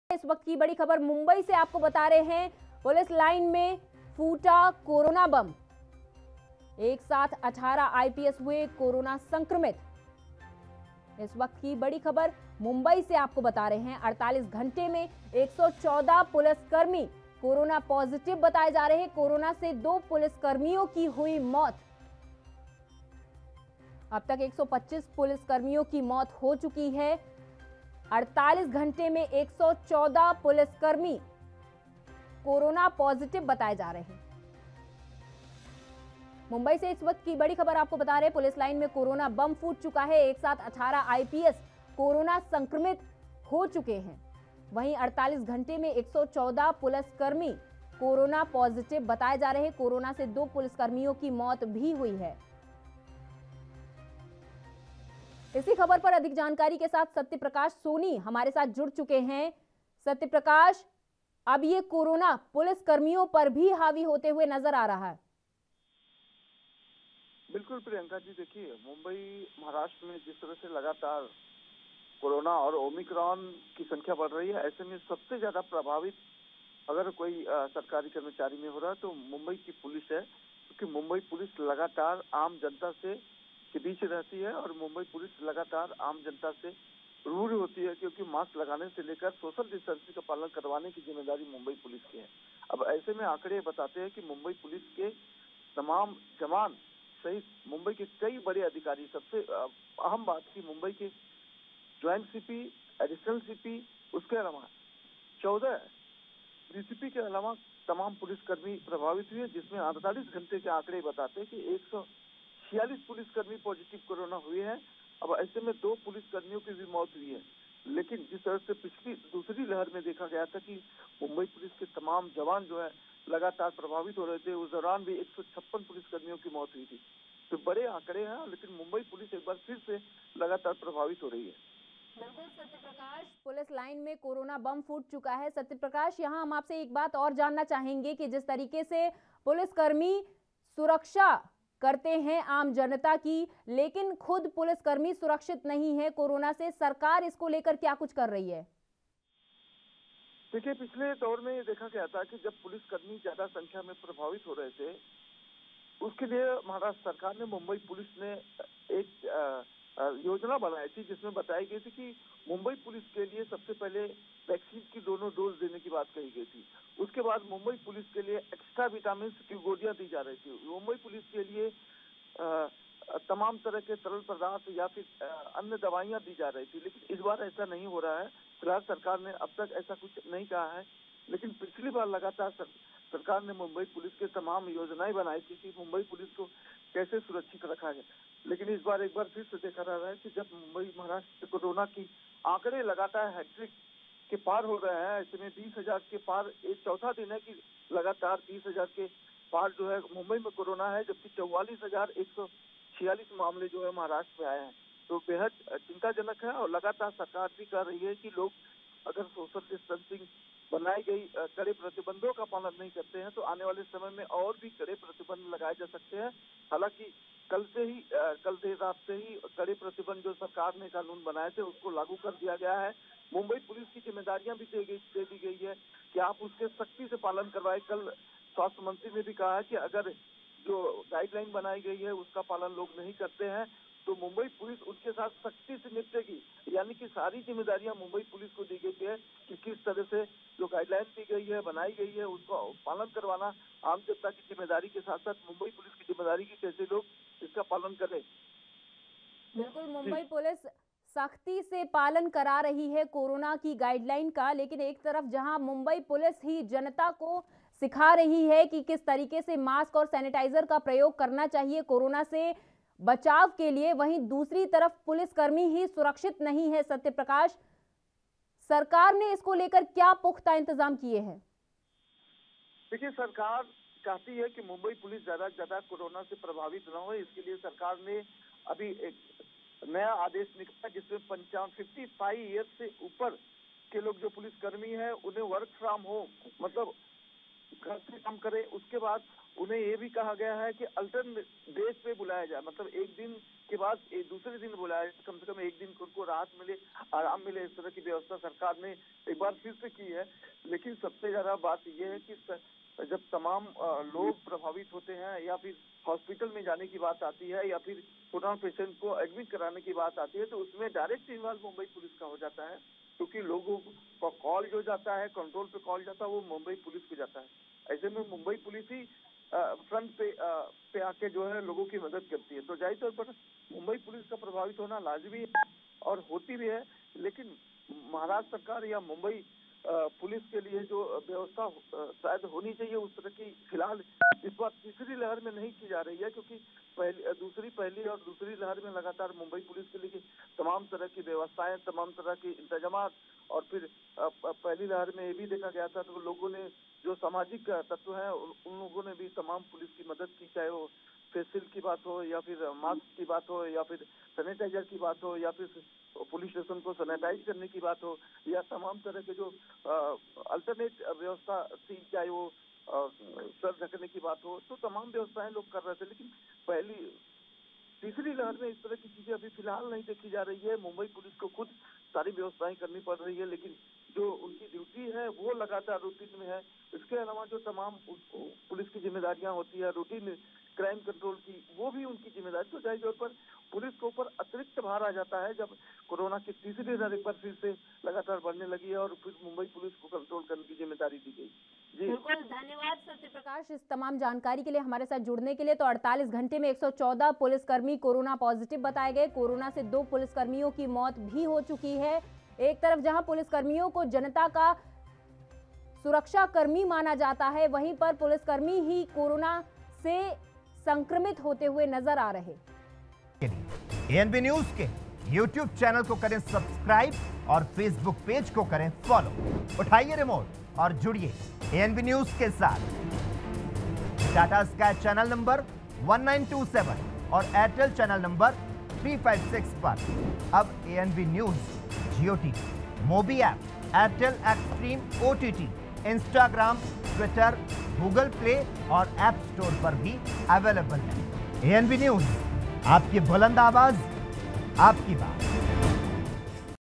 एक साथ 18 IPS हुए कोरोना संक्रमित | ANB NEWS
मुंबई- पुलिस लाइन में फूटा कोरोना बम एक साथ 18 IPS हुए कोरोना संक्रमित
48 घंटे में 114 पुलिसकर्मी कोरोना पॉजिटिव  कोरोना से 2 पुलिसकर्मियों की हुई मौत
अब तक 125 पुलिसकर्मियों की मौत
Mumbai police line#police#corona positive#corona death#covid-19#breakingnews